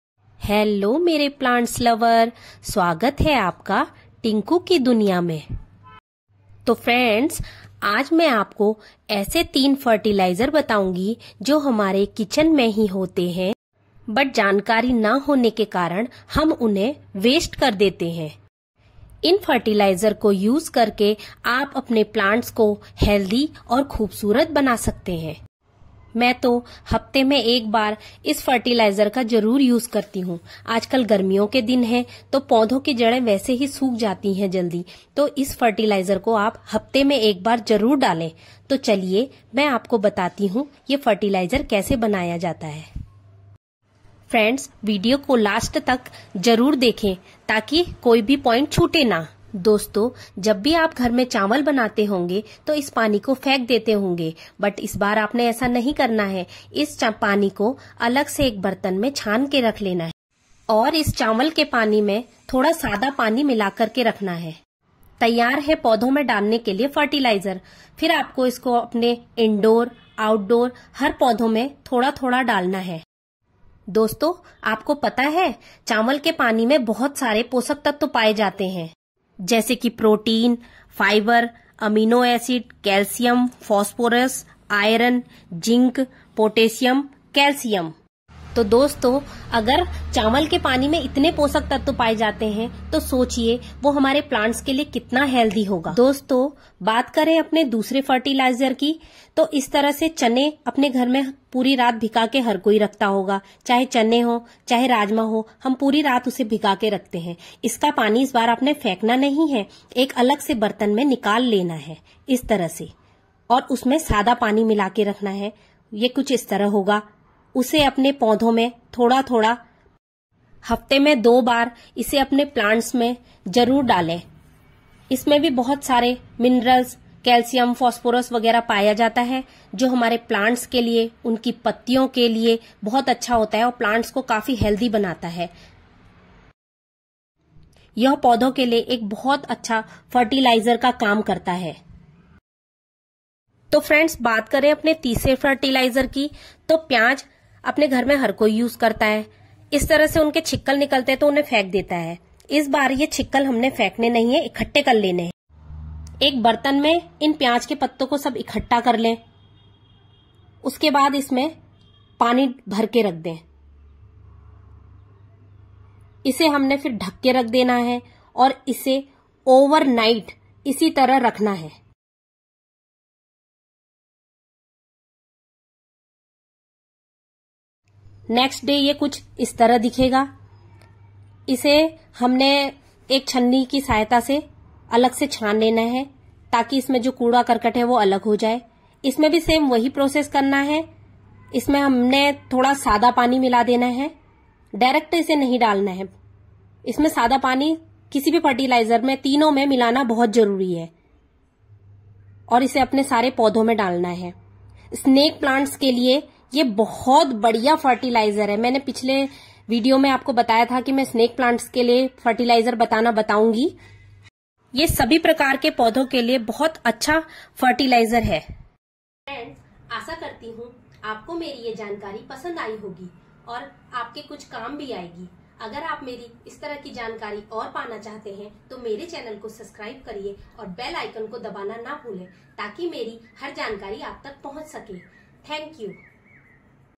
किचन वेस्ट से फार्टिलाइजर बनाना जो पौधों में ला देगा जान / best fertilizer / kitchen fartilizer
किचन वेस्ट फर्टिलाइजर